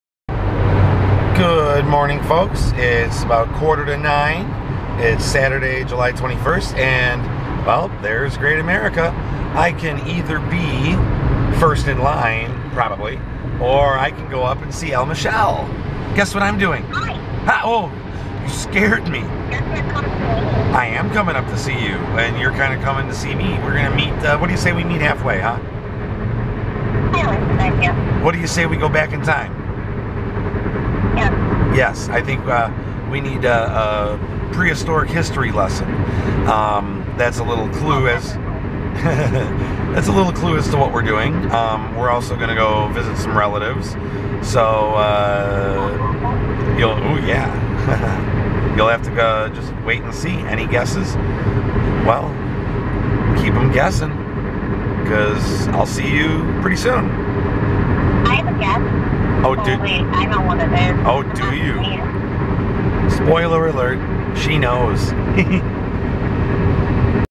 Passing Six Flags Great America Gurnee, IL 7-21-18
On the way up to see L. Michelle, I did another video passing the park and anticipating future plans!